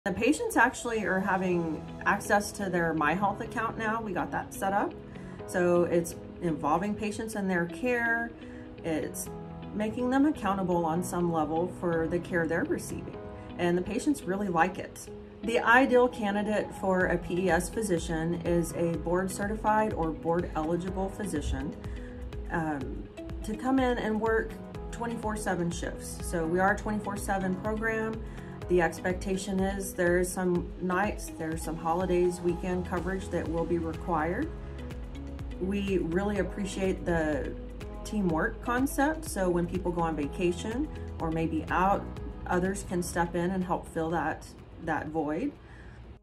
Being a PES at The Harris Center
The Harris Center makes every effort to provide high-quality services so that persons with mental disabilities may live with dignity and be fully functioning members of our community.
To provide Psychiatric Emergency Service, we need board-certified and board-eligible physicians to come in and work 24/7 shifts and handle the teamwork concept. Amber Pastusek, the PES Medical Director of Neuropsychiatric Center, openly relates the ideal candidate for a PES position and invites you to apply.
To be a part of these exciting career opportunities, visit our